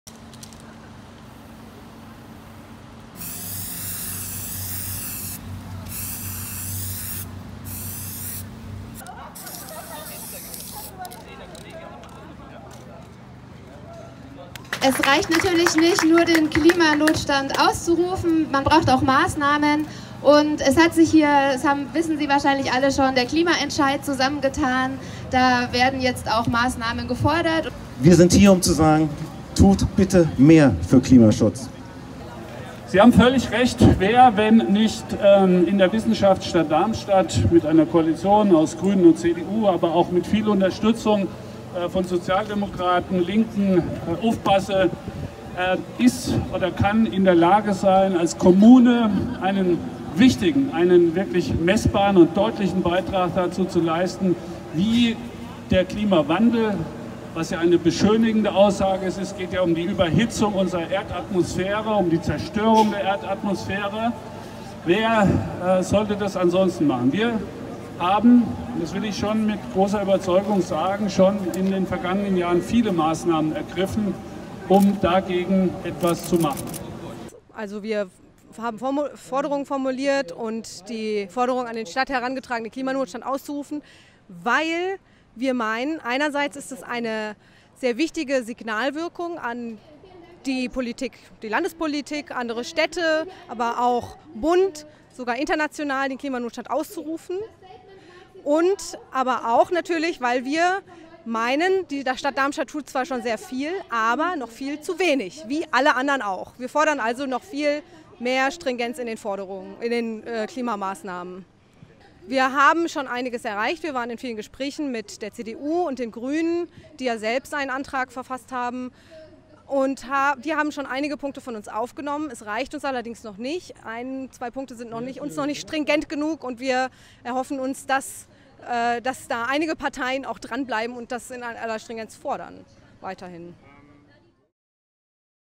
Resolution zu Klimanotstand an OB Partsch übergeben
Die Bürger*innen-Initiative Klimanotstand Darmstadt hat eine Resolution an die Vertreter der Stadt offiziell übergeben. Darin fordert die initiative die Stadt Darmstadt auf, mehr für den Klimaschutz in Darmstadt zu übernehmen.